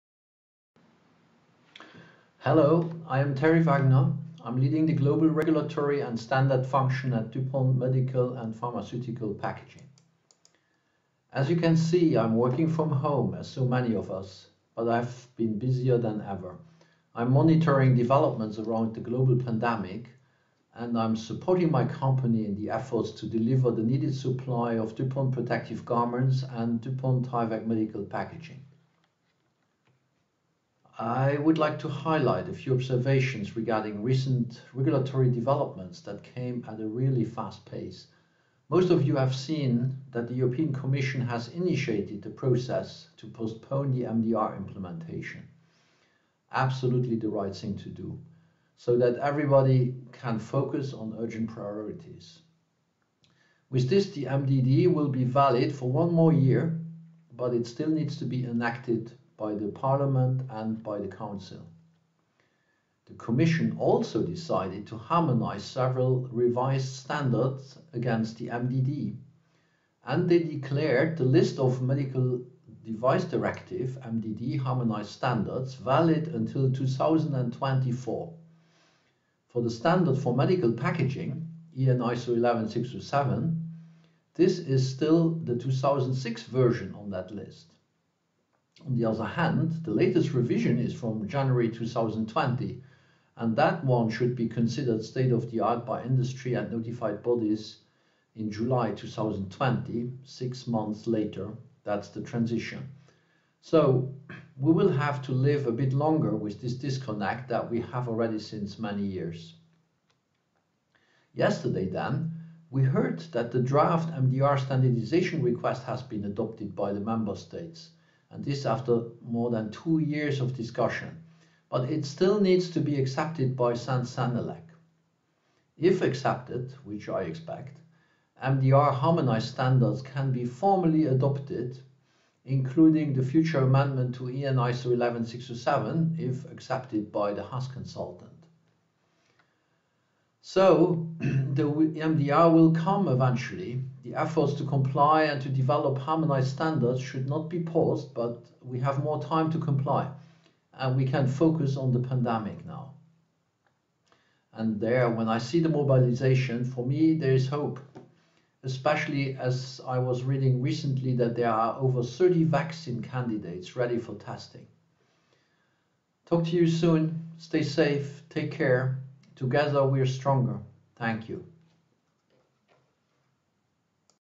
Medical Packaging Regulatory & Standards Update (March 31, 2020)
Thierry Wagner, Global Regulatory & Standards Leader at DuPont™ Tyvek® Medical & Pharmaceutical Packaging, provides the latest update regarding recent regulatory updates from the European Commission about MDR, MDD, ISO-11607 and more.

Video was recorded on March 31, 2020.